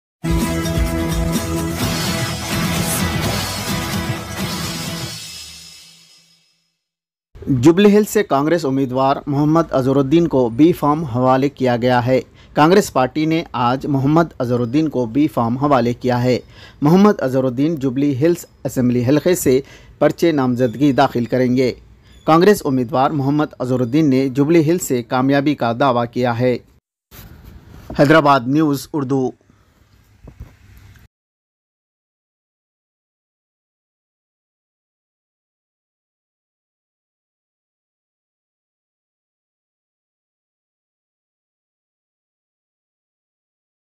Congress Candidate From Jubilee Hill Form Azharuddin Gets B form & He Will File Nomination Soon
Congress  Candidate  From Jubilee Hill Form Mohammed  Azharuddin Gets B form & He Will  File Nomination  Soon